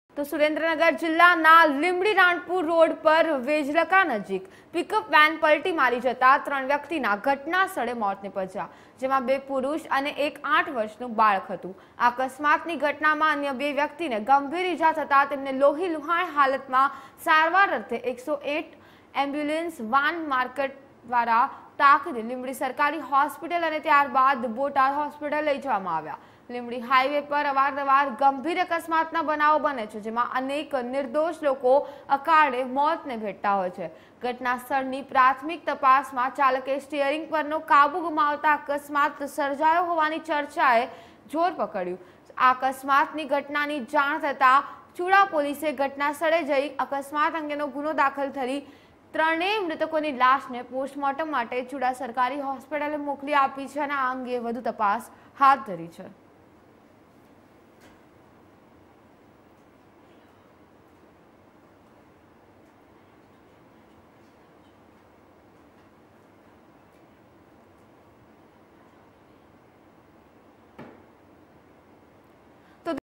Accident In Surendranagar, 3 Dead :પિકઅપ વાન પલટી, ત્રણ લોકોના મોત- Latest Gujarat News
સુરેન્દ્રનગર જિલ્લાના લીંબડી-રાણપુર રોડ ઉપર વેજલકા નજીક પિકઅપ વાન પલટી મારી જતાં ત્રણ વ્યક્તિનાં ઘટનાસ્થળે મોત નીપજ્યાં હતાં, જેમાં બે પુરુષ અને એક આઠ વર્ષનું બાળક હતું. આ અકસ્માતની ઘટનામાં અન્ય બે વ્યક્તિને ગંભીર ઇજા થતાં તેમને લોહીલુહાણ હાલતમાં સારવાર અર્થે 108 એમ્બ્યુલન્સ વાન દ્વારા તાકીદે લીંબડી સરકારી હોસ્પિટલ અને ત્યાર બાદ બોટાદ હોસ્પિટલ લઇ જવામાં આવ્યા હતા. લીંબડી હાઇવે પર અવારનવાર ગમખ્વાર અકસ્માતના બનાવો બને છે, જેમાં અનેક નિર્દોષ લોકો અકાળે મોતને ભેટતા હોય છે. ઘટનાસ્થળની પ્રાથમિક તપાસમાં ચાલકે સ્ટીયરિંગ પરનો કાબૂ ગુમાવતાં અકસ્માત સર્જાયો હોવાની ચર્ચાએ જોર પકડ્યું છે. સર્જાયેલી આ અકસ્માતની ઘટનાની જાણ થતાં ચૂડા પોલીસે ઘટનાસ્થળે પહોંચી અકસ્માત અંગેનો ગુનો દાખલ કરી ત્રણેય મૃતકોની લાશને પોસ્ટમોર્ટમ માટે ચૂડા સરકારી હોસ્પિટલે મોકલી આપી વધુ તપાસ હાથ ધરી હતી.